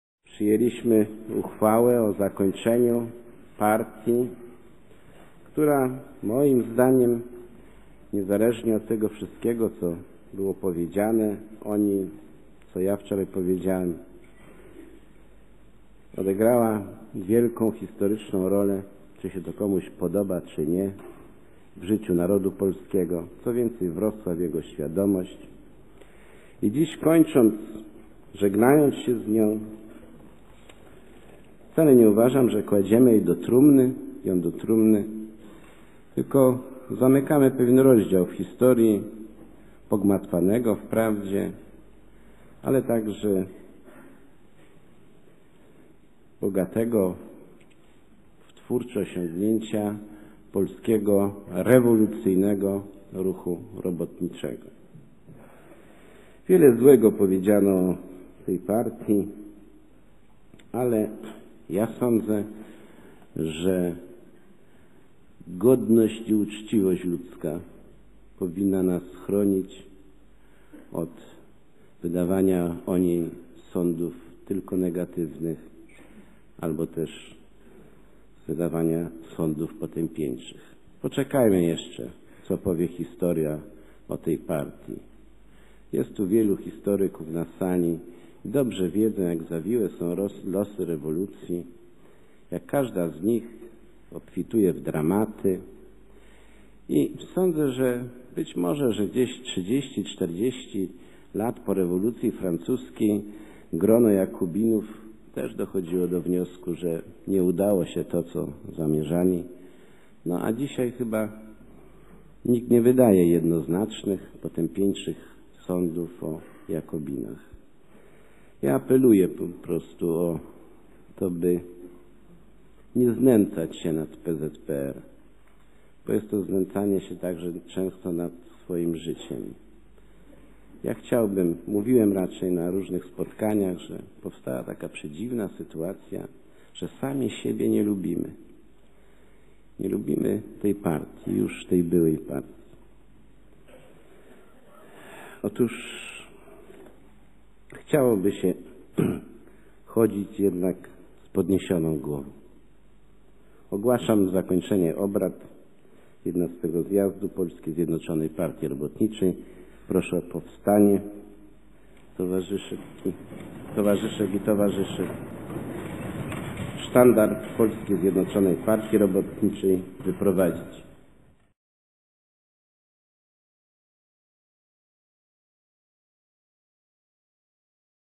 Ostatni zjazd PZPR- Rakowski „sztandar PZPR wyprowadzić”
Plakaty propagandowe z okresu PRL + nagranie z XI zjazdu PZPR